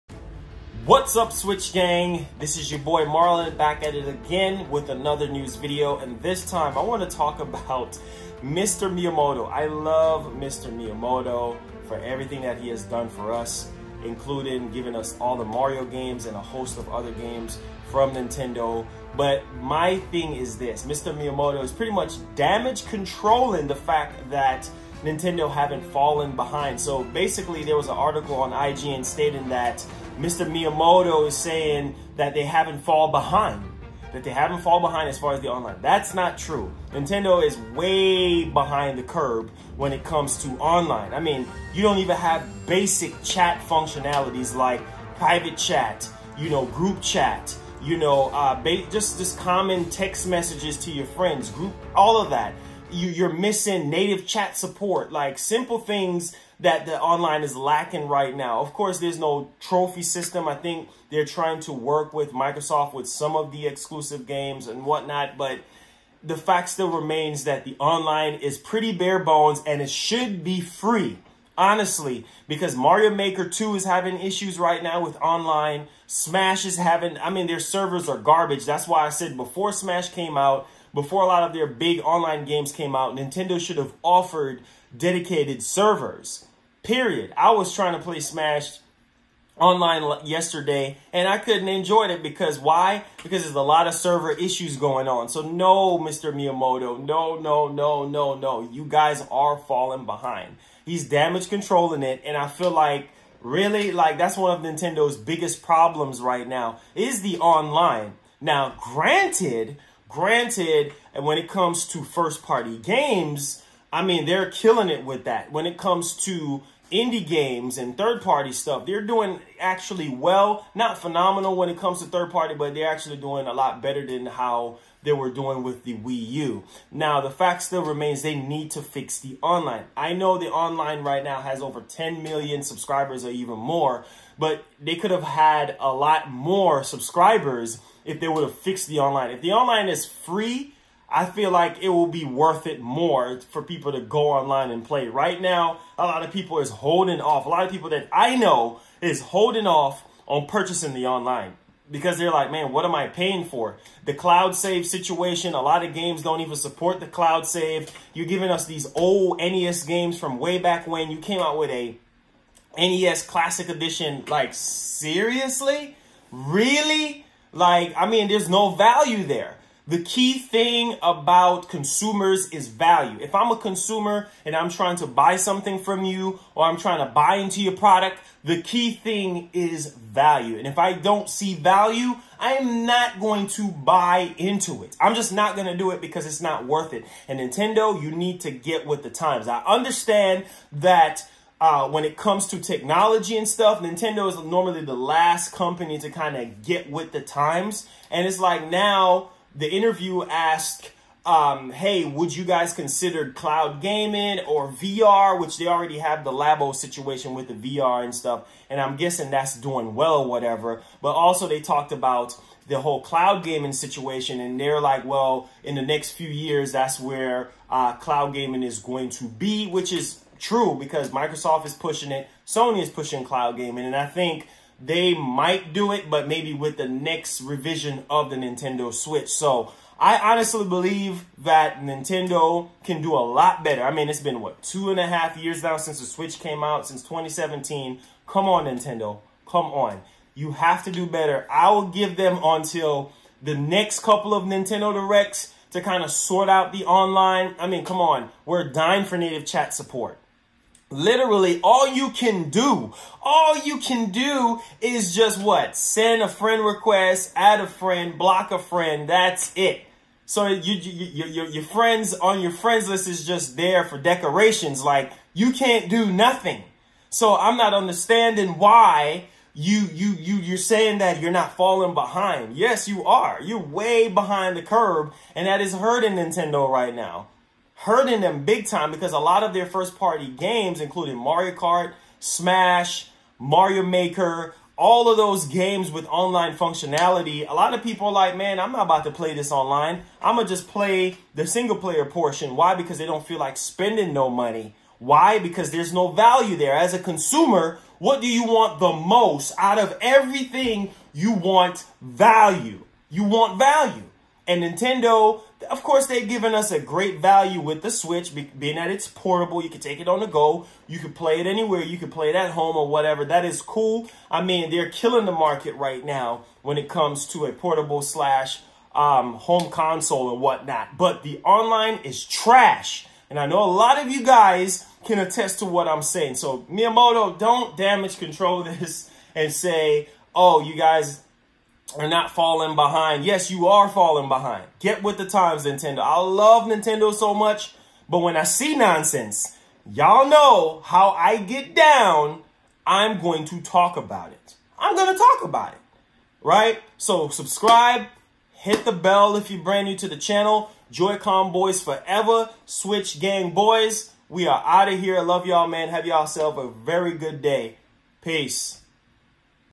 Miyamoto Damage Control Switch Online Rant!!! (Nintendo Hasn’t fallen Behind)
Nintendo Has 'Not Fallen Behind' On Online Network Services, Says Miyamoto Switch online should be free because it lacks basic communication features and there is just no value whatsoever.